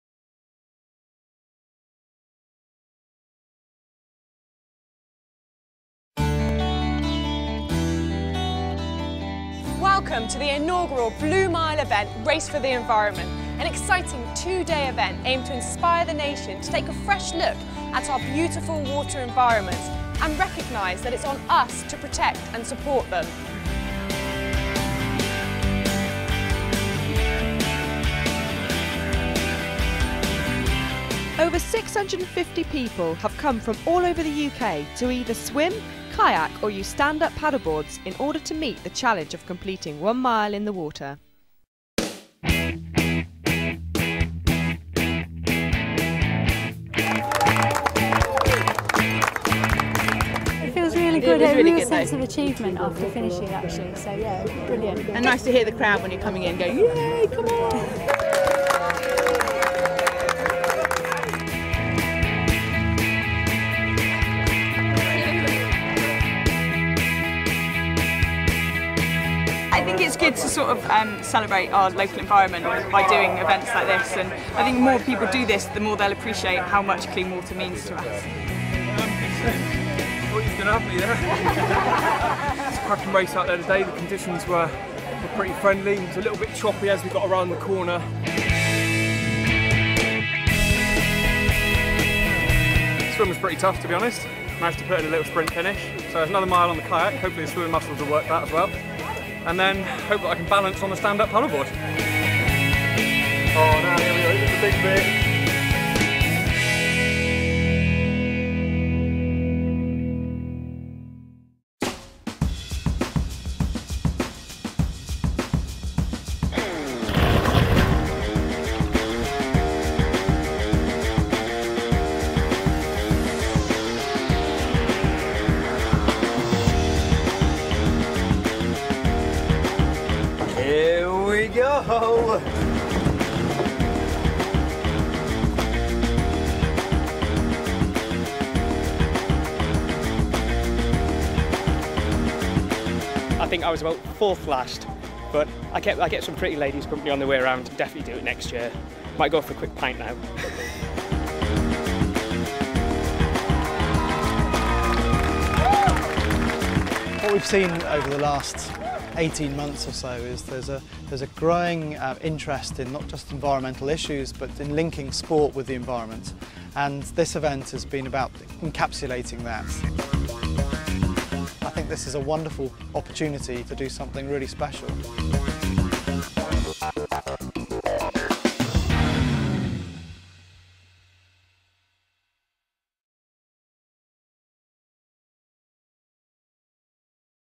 The Blue Mile - Race for the Environment
The Blue Mile - Race for the Environment, the UK's newest mass-participation event designed to get the nation active and enjoying our blue environment,